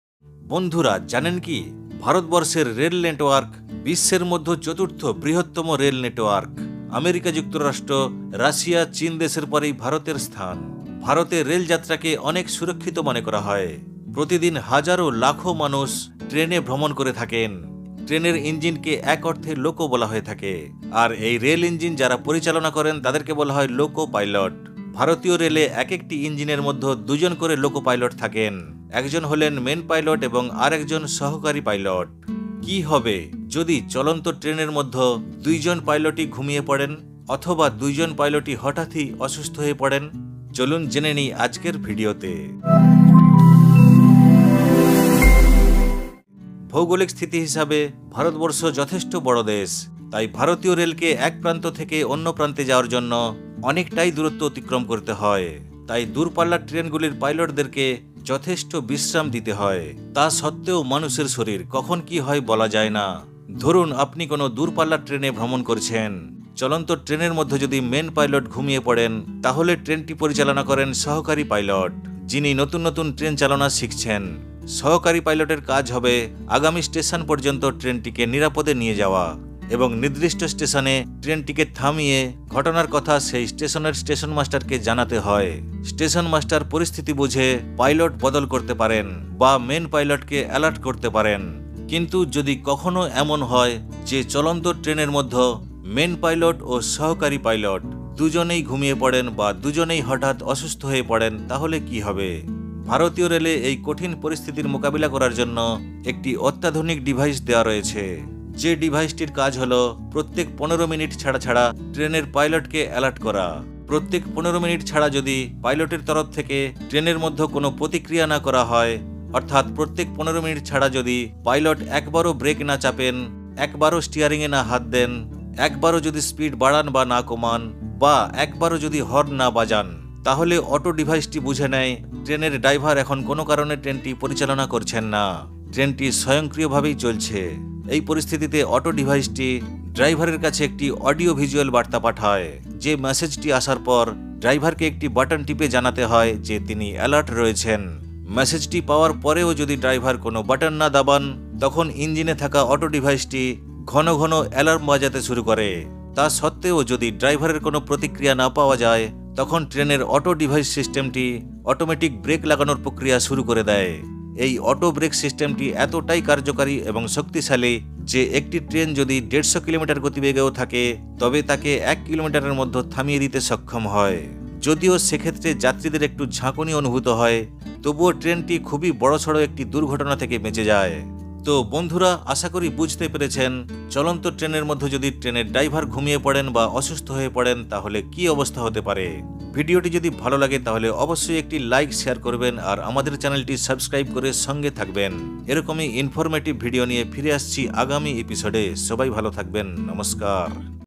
চলন্ত ট্রেনের চালক ঘুমিয়ে পড়লে কি ঘটবে | What happened if Train Loco Pilot will sleep
মানুষ ছাড়া পৃথিবী কেমন হবে

In India Rail network is worlds no 4th position.Manu people travel in train.What happened if the train Driver is sleeping in runing train.